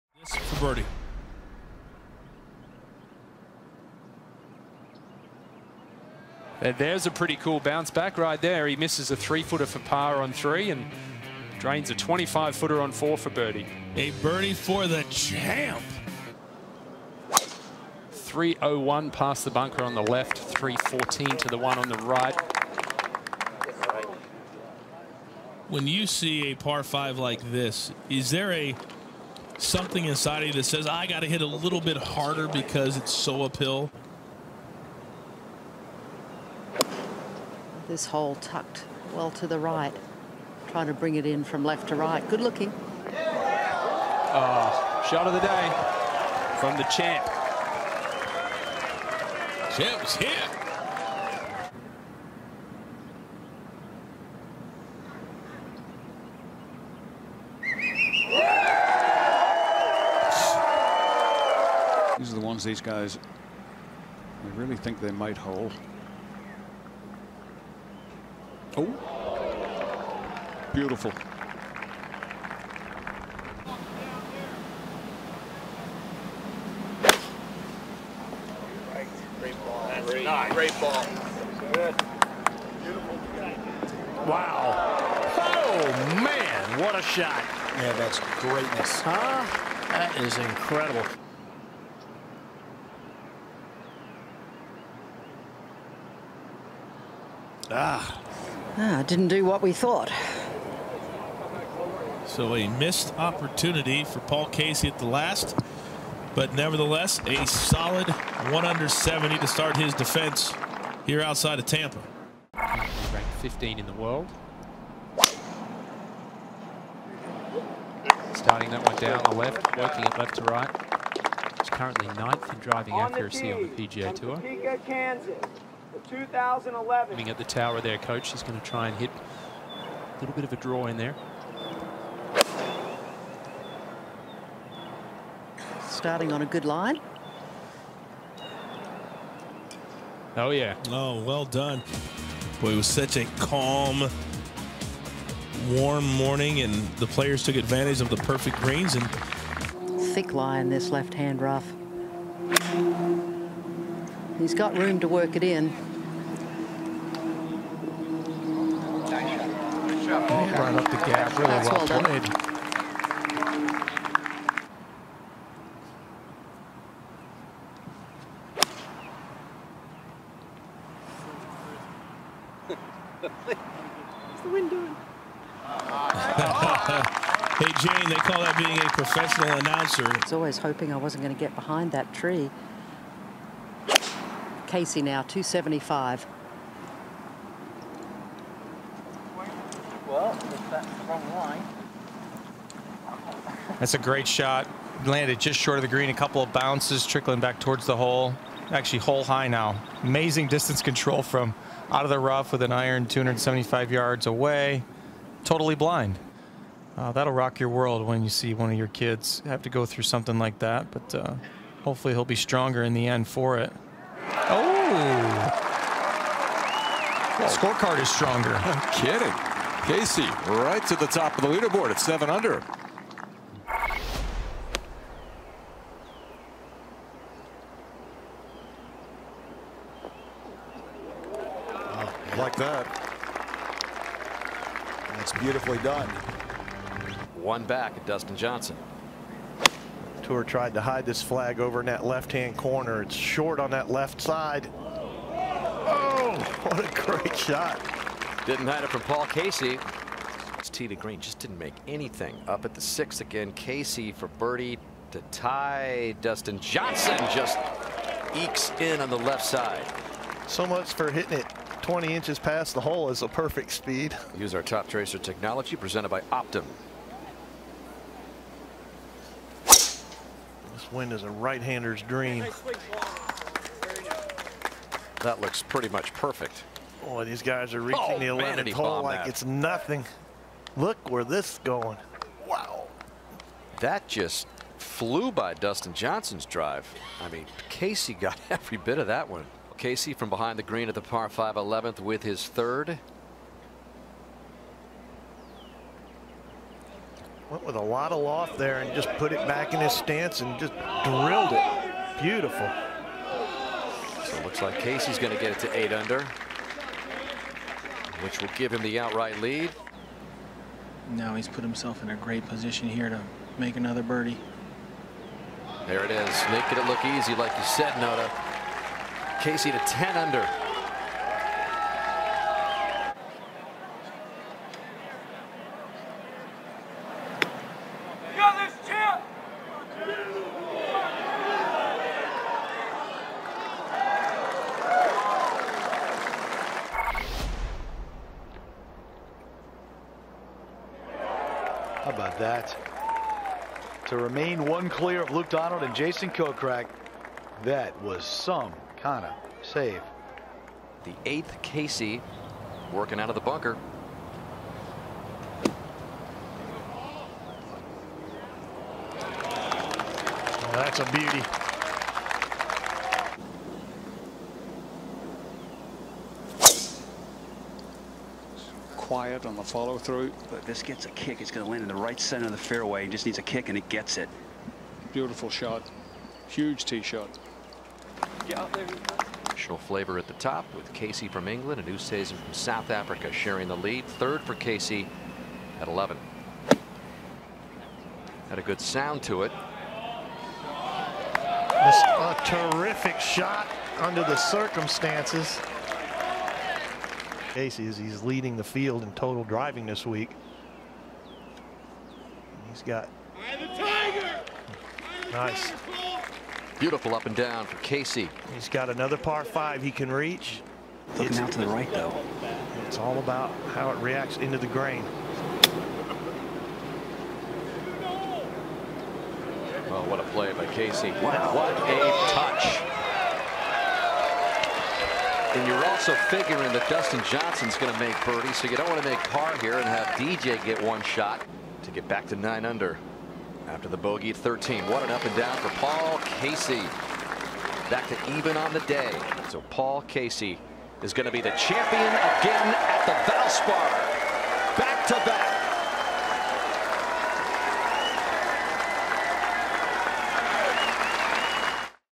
Paul Casey’s winning highlights from Valspar 2019
In the 2019 Valspar Championship, Paul Casey claimed his third victory on the PGA TOUR with rounds of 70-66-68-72 to finish at 8-under par at Innisbrook.

The Valspar Championship is held on the Copperhead Course at Innisbrook Resort and Golf Club in Palm Harbor, Florida. In 2018, Paul Casey closed with a 6-under 65 and won the tournament as Tiger Woods came up one putt short of forcing a playoff.